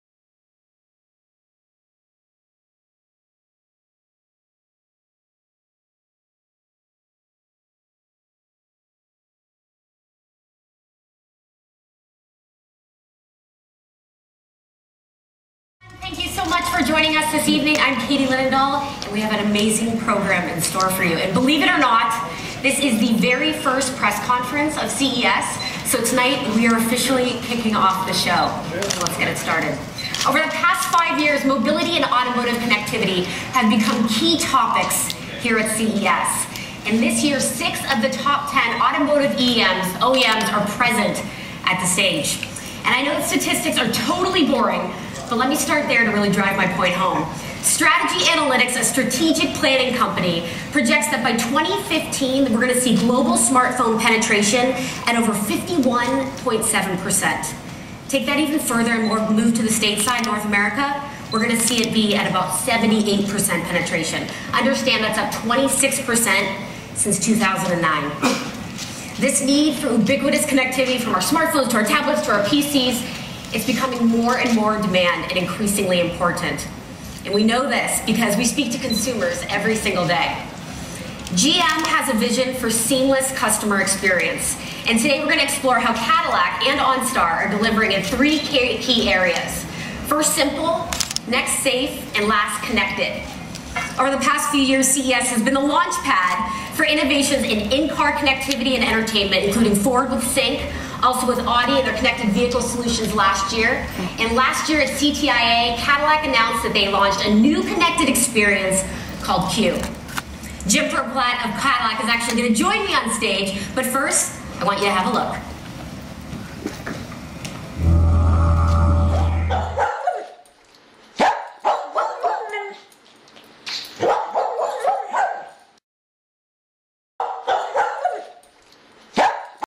Katie Linendoll for Onstar at the Onstar press conference at the 2012 International CES in 3D
Katie Linendoll for Onstar at the Onstar press conference at the 2012 International CES in anagryphl 3D,